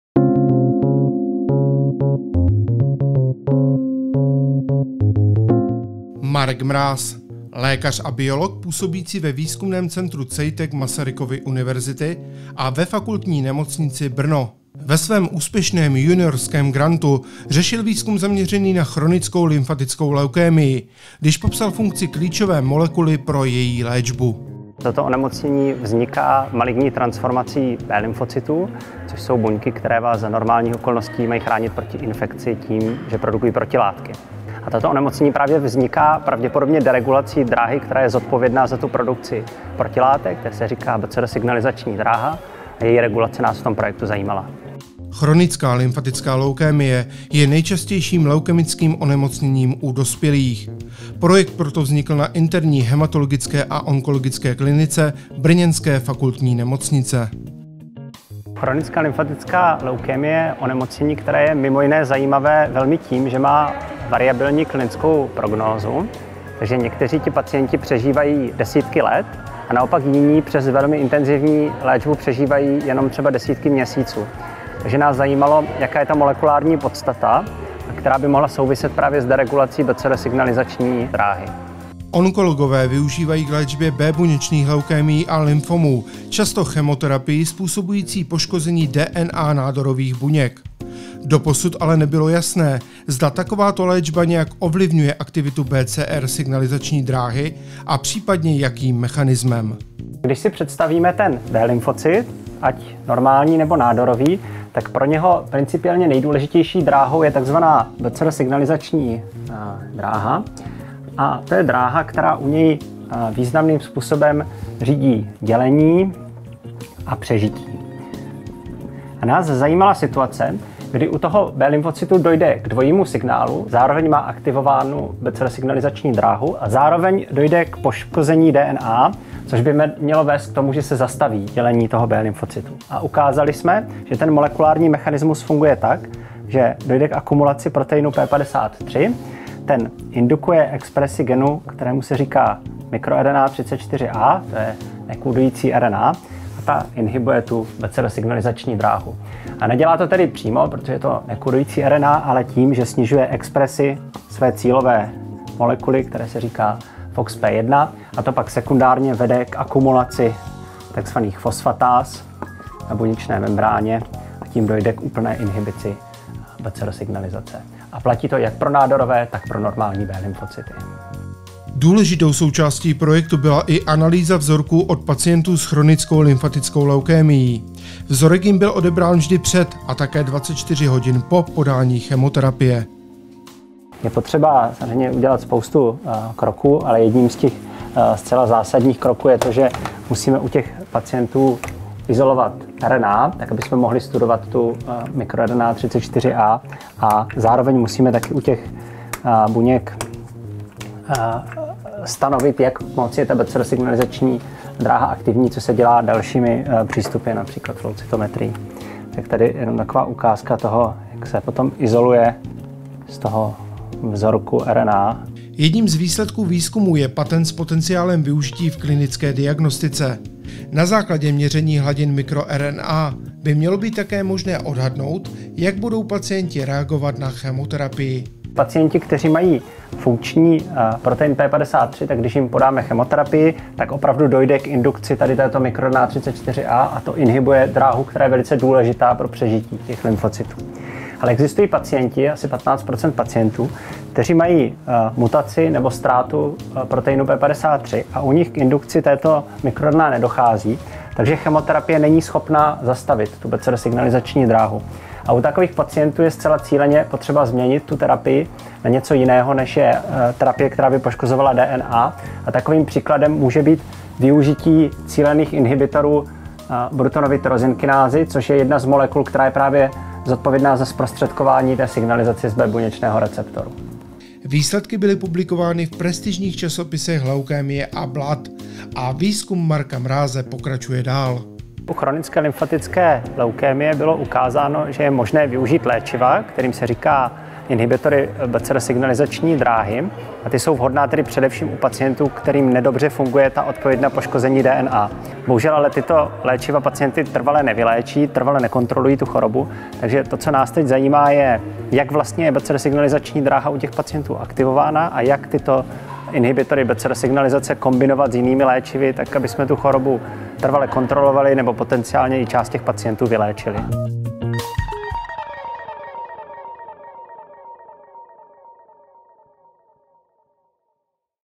Marek Mráz, laureát Ceny předsedkyně Grantové agentury ČR za rok 2019
Cena předsedkyně za projekt "Regulace signalizační dráhy B-buněčného receptoru (BCR) prostřednictvím microRNA"
doc. MUDr. Mgr. Marek Mráz, Ph.D.
FN Brno - Interní hematologická a onkologická klinika a Masarykova univerzita – Lékařská fakulta; CEITEC Masarykova univerzita

Laboratoř doc. Mráze se věnuje studiu B buněčných leukémií a lymfomů, což jsou onemocnění vznikající z B lymfocytů. Ke vzniku těchto nádorů často vede porušení molekulární dráhy, která řídí produkci protilátek u B lymfocytů tzv. signalizace přes B buněčný receptor (BCR). V oceněném projektu bylo úplně poprvé popsáno, že existuje mechanismus, jímž maligní B lymfocyty utlumí aktivity BCR dráhy v momentě, kdy je poškozena jejich DNA. Objevený mechanismus zahrnuje protein p53 a tzv. nekódující RNA miR-34a a její cílové molekuly. Měření hladiny miRNA lze také použít k predikci léčebné odpovědi u pacientů s chronickou lymfatickou leukémií (CLL) a B lymfomy. Pacienti bez funkční p53 dráhy (cca 15 % CLL pacientů) mají aktivní BCR signalizaci i přes podání chemoterapie a je u nich velmi žádoucí zvolit jinou léčbu např. přímé BCR inhibitory.